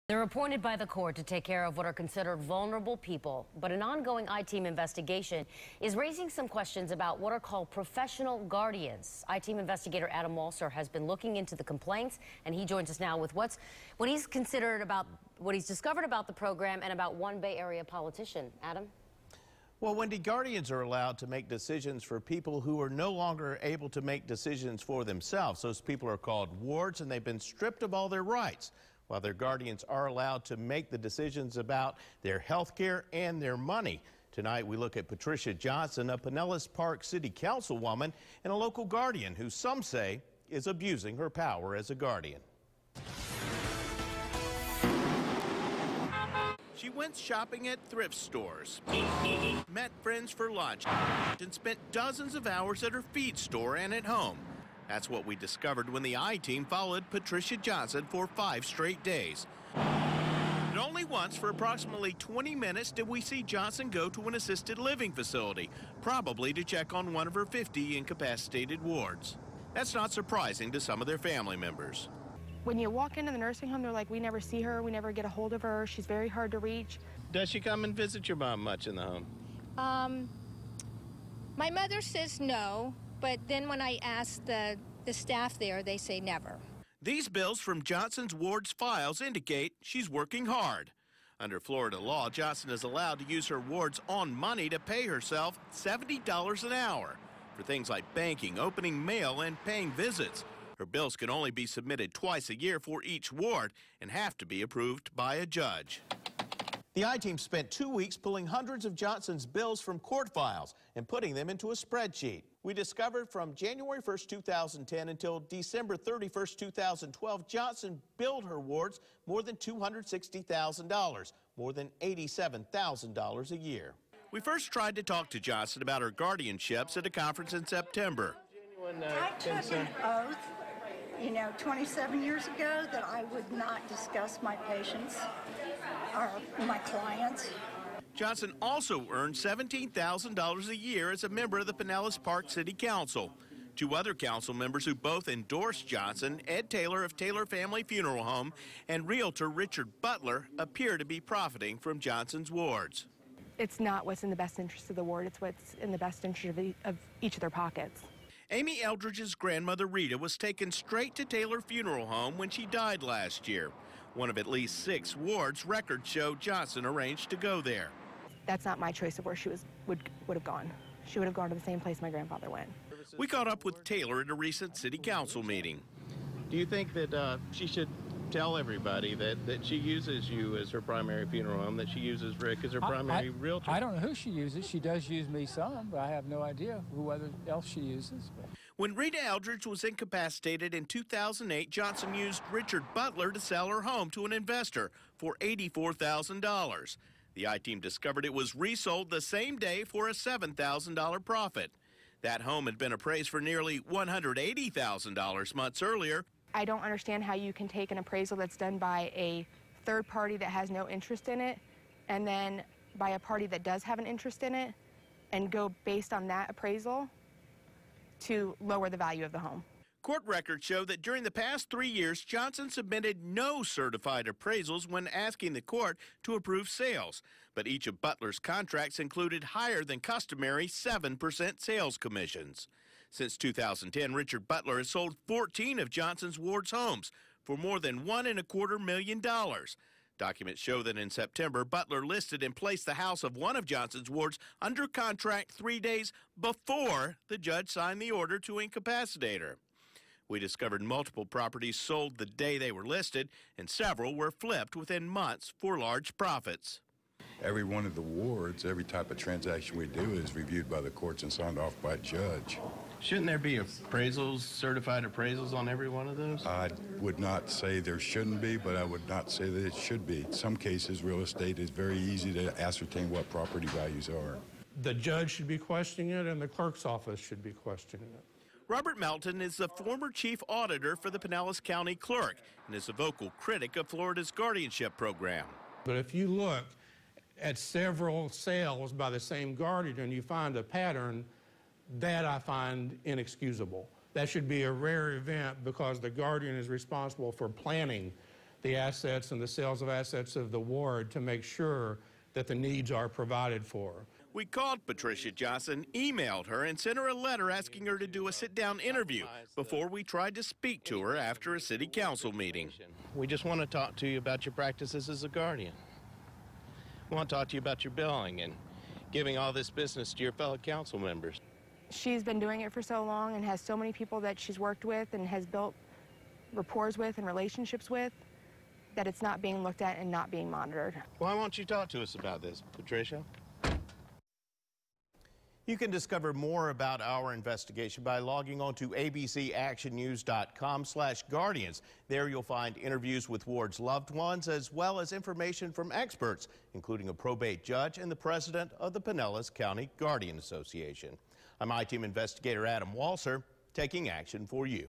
Losing Freedom: The I-Team Investigates Florida's Guardianship Program
She met friends for lunch, went shopping at thrift stores and spent dozens of hours at her feed store and at home.